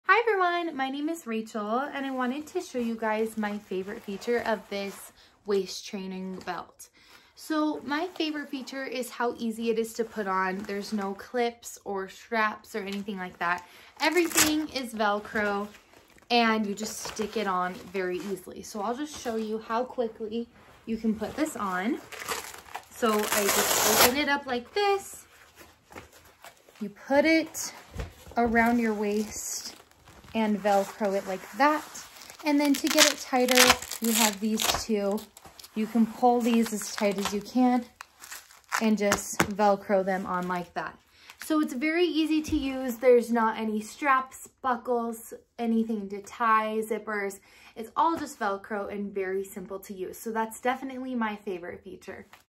Waist Trainer Belt Review: The Truth Exposed
Curious about waist trainer belts? Watch this review of the ChongErfei Waist Trainer Belt for Women and Men to discover the truth about this waist trimmer and weight loss ab belt.

ChongErfei Waist Trainer Belt for Women Man - Waist Trimmer Weight Loss Ab Belt - Slimming Body Shaper